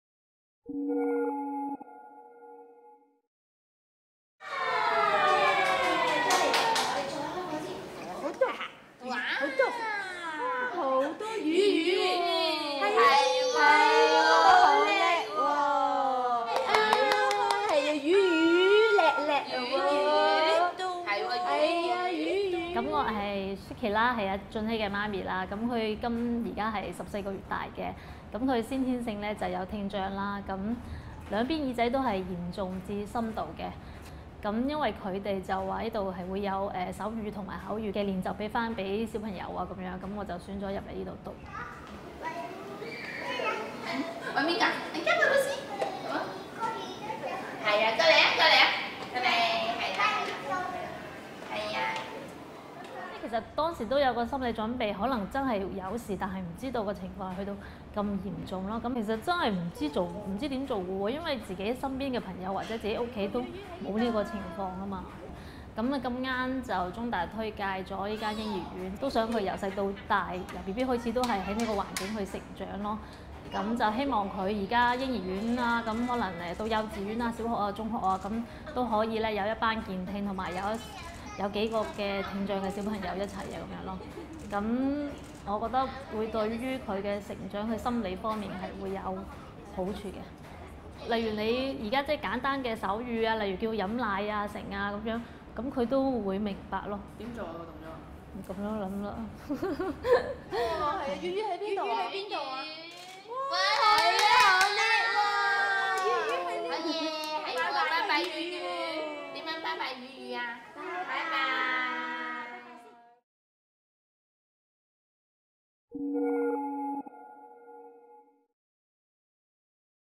Baby sign language taught in Hong Kong Society for the Protection of Children
Private day care centre incorporates sign language in their classes for 0-2 years old, taking in their first hearing-impaired baby this year and about to add in a second on October. Parents and school reflect on how adding in sign language benefits all babies, during the most crucial learning time for children – an age group ignored by the Education Bureau and receives literally no support from the government.

This video was originally published on 5 September 2014